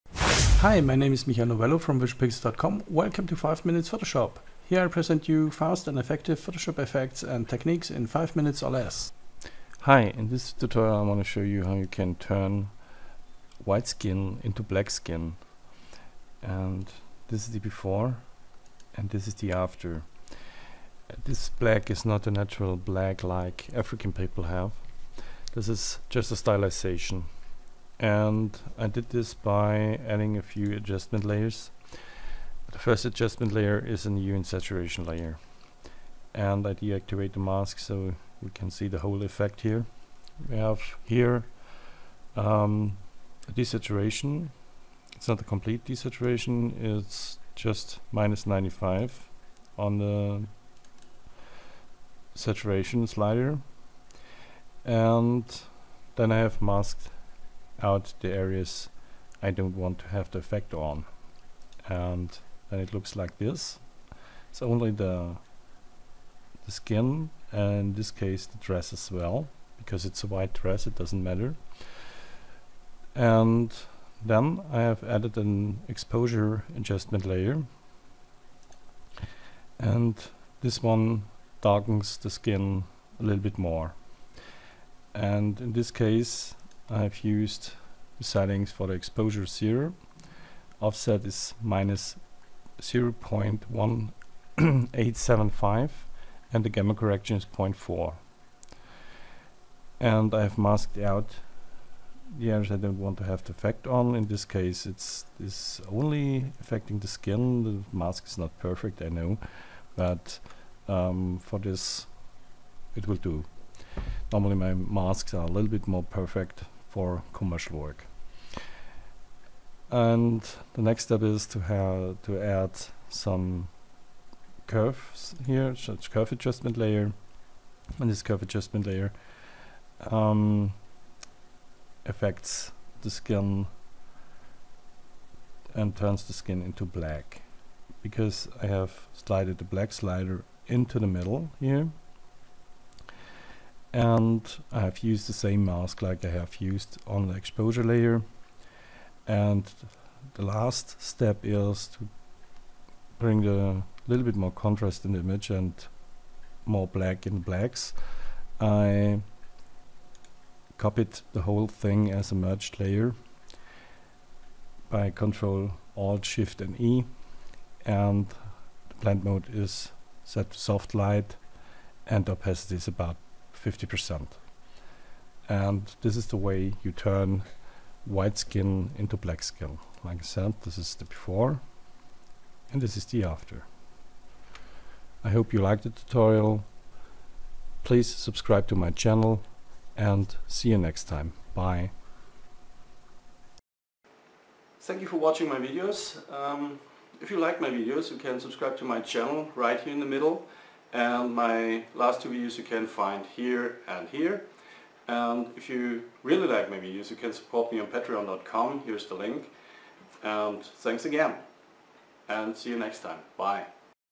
Photoshop Tutorial: How to Create Black Skin
In this Photoshop tutorial I show you how to turn white caucasian Skin into black skin.

My Websites:
Visual-Pixels - all about Digital-Art